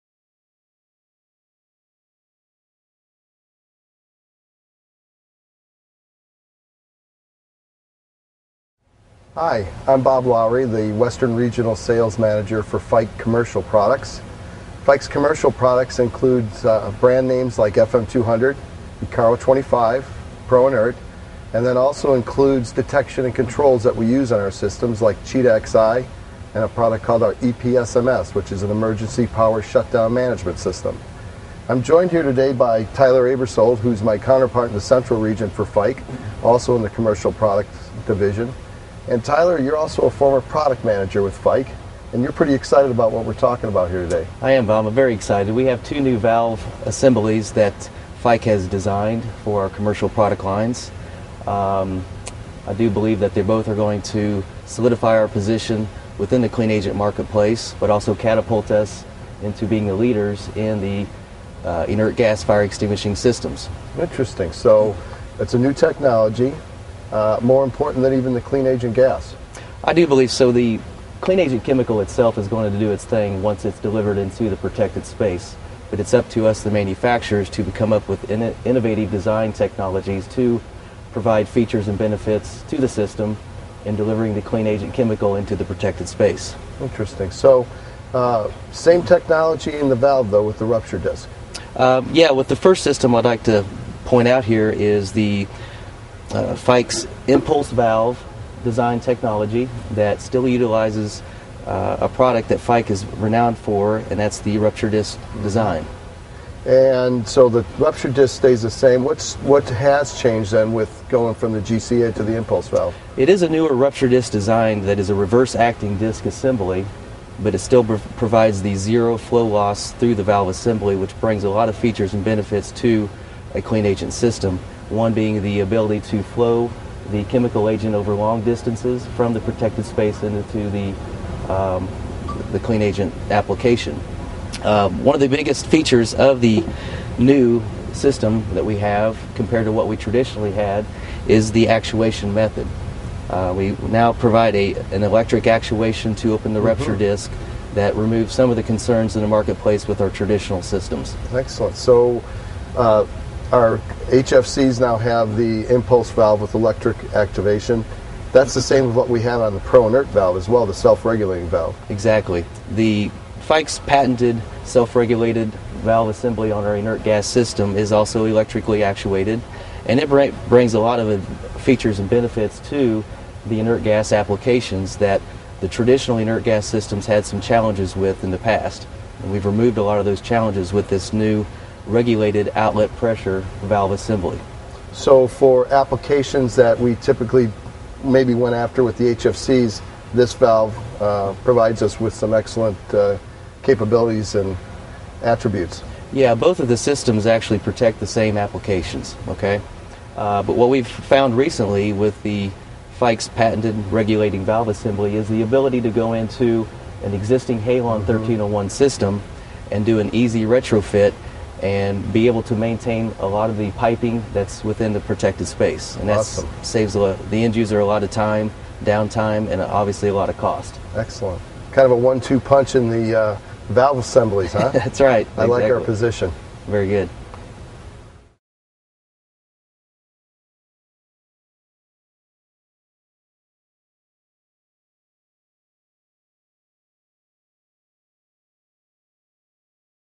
Fike Fire Suppression Solutions
Known for quality, dependable fire protection systems, Fike continues to look for ways to improve the effectiveness of all its safety products. This video features Fike's fire suppression valve technologies:  the Impulse Valve for superior  performance in clean agent fire suppression systems; and the unique ProInert valve for the safest, most economical  inert gas fire protection system available.